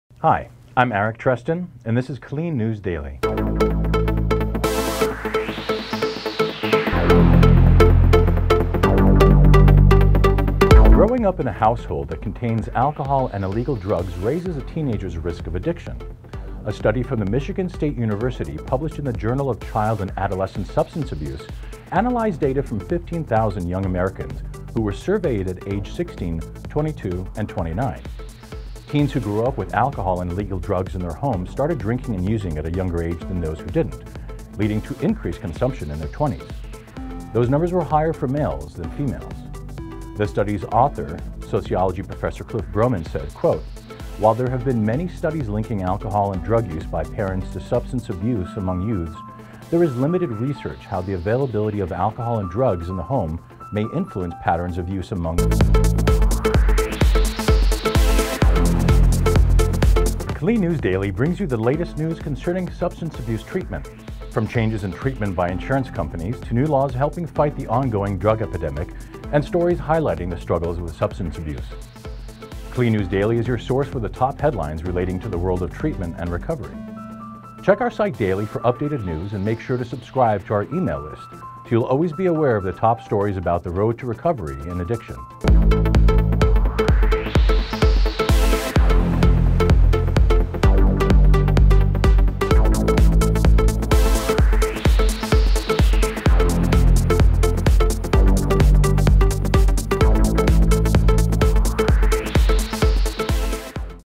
Keep illicit drugs, booze out of the home to lower teens' addiction risk
Growing up in a household that contains alcohol and illegal drugs raises a teenager’s risk of addiction. A study from Michigan State University published in the Journal of Child & Adolescent Substance Abuse, analyzed data from 15,000 young Americans who were surveyed at age 16, 22, and 29. Teens who grew up with alcohol and illegal drugs in their homes started drinking and using at a younger age than those who didn’t, leading to increased consumption in their 20’s. Those numbers were higher for males than females. The study’s author sociology professor Cliff Broman said, “while there have been many studies linking alcohol and drug use by parents to substance use among youths, there is limited research how the availability of alcohol and drugs in the home may influence patterns of use among offspring in the future.”